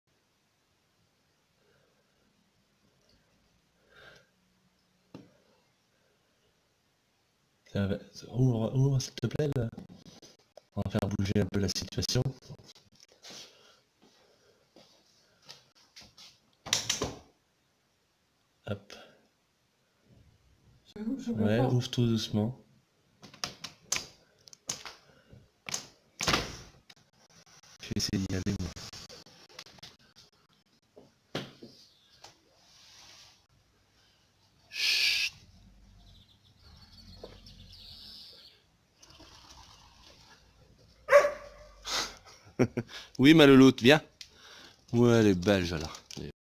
Je t'aurai un jour, je t'aurai...... de munster de la vallée de la Vaunoise !
Jalla en arrêt sur un pigeon ramier dans notre jardin est filmée depuis notre cuisine avec mon téléphone.